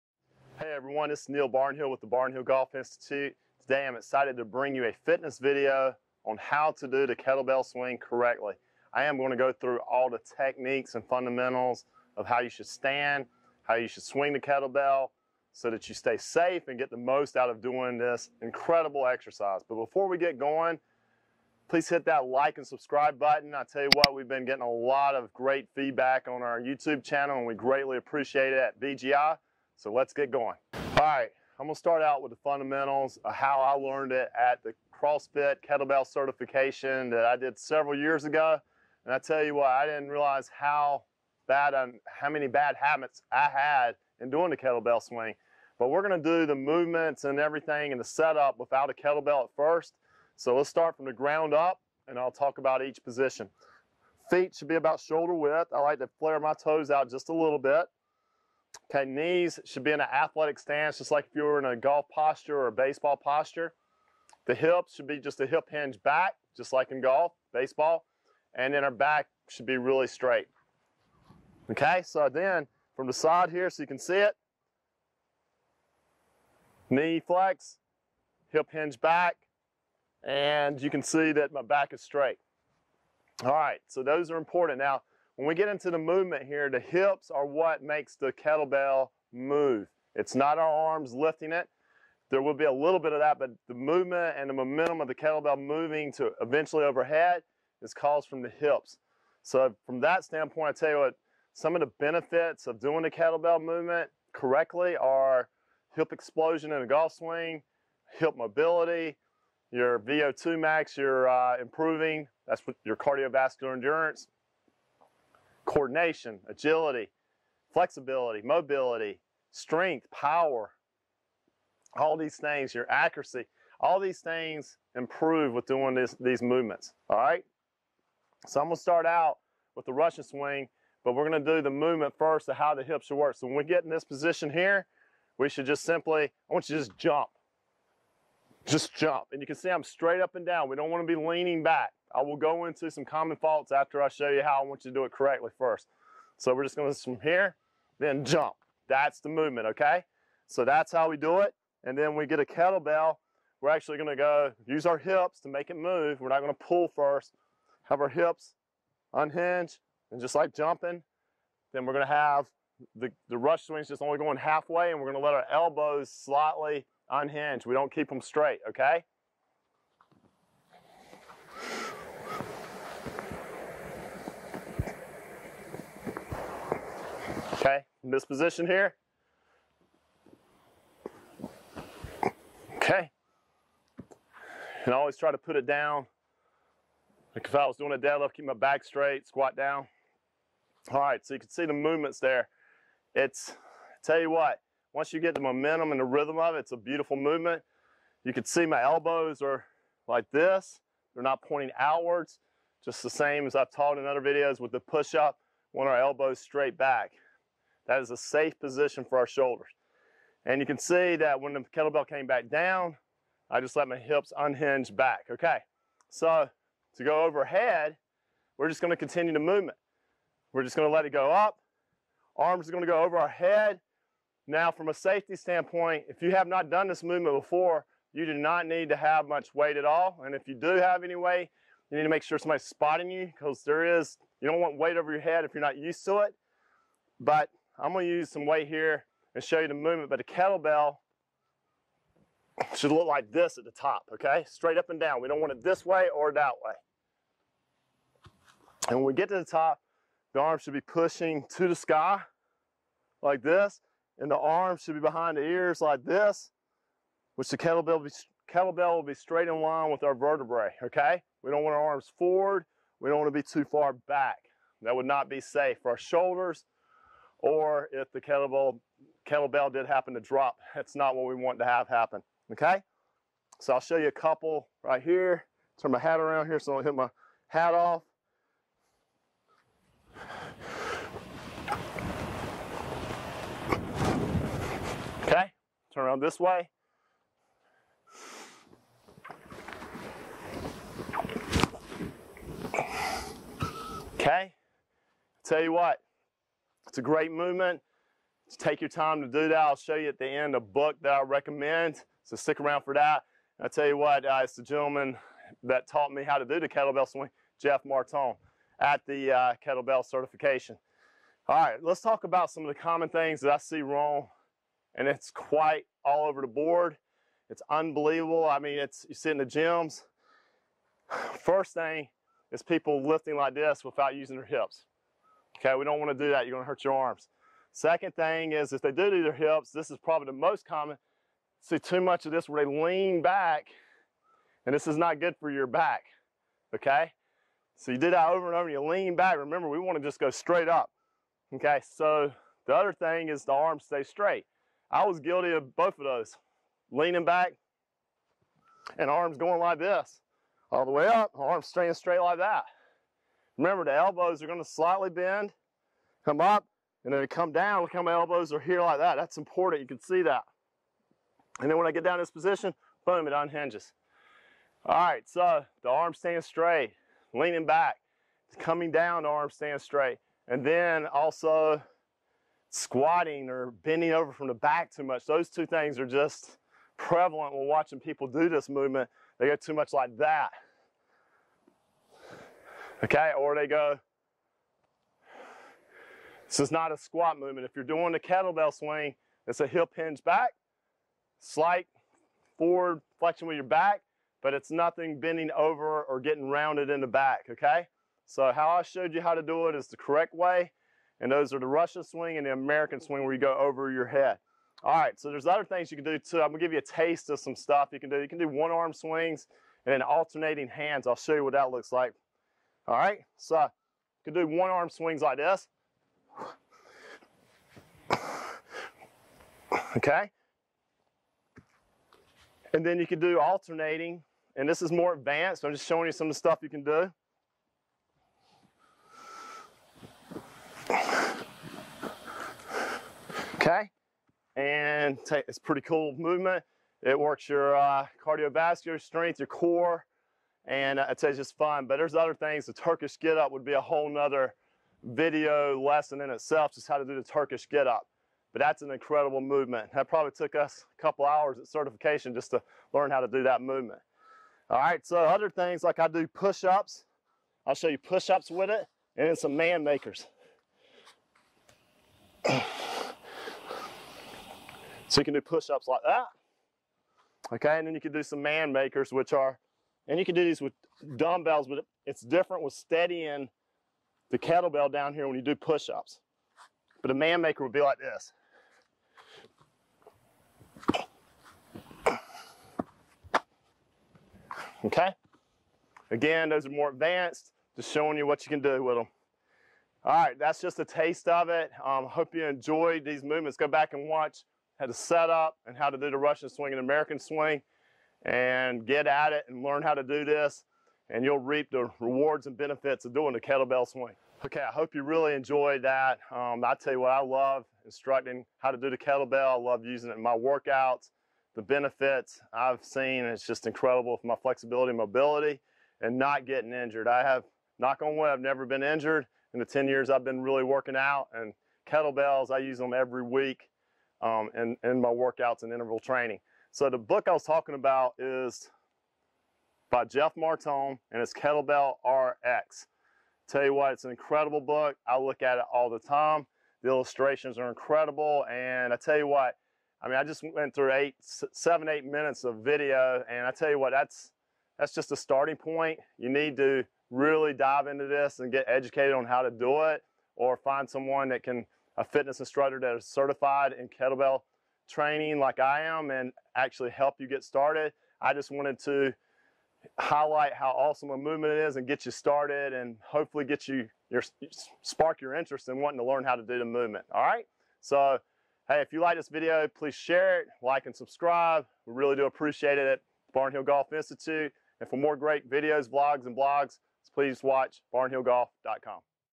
Using Kettlebells the Right Way - Golf Fitness
Neil Barnhill, certified CrossFit and Kettle certified instructor, shows you how to use kettlebells the right way to build strenght leading to lower scores and more fun on the golf course.

Welcome to BGI, Neil Barnhill, PGA Teaching Professional, and Kyle Clapper are committed to providing the highest quality golf content.